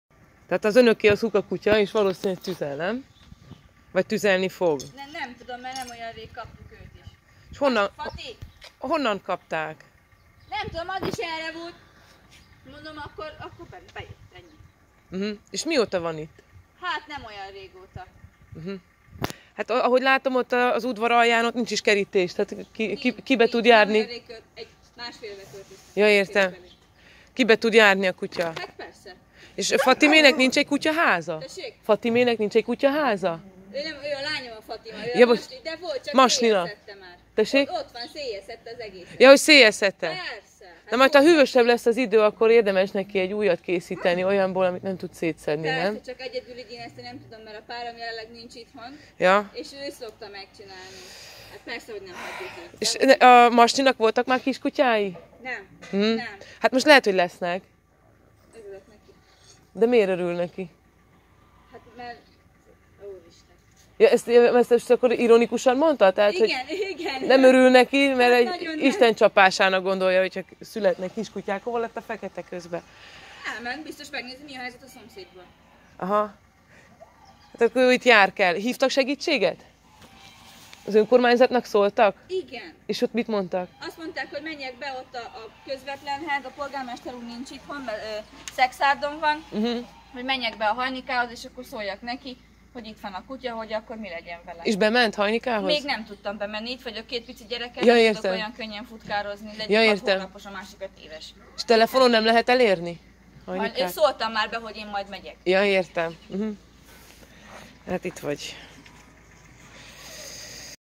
Kóborkutya Gyulajon - Straydog in Gyulaj - Hungary
Az udvaron láncra kötött tüzelő szuka kutyához, odaszokott egy kóbor kan kutya. Eleinte az embereket nem engedte ki a házból, később barátságosabb ebbé vált. Az utcában egyébként még legalább nyolc ugyanilyen keverék szuka kutyát láttam.
Senki, de senki nem tudott a helyzeten segíteni, mert nincs férőhely.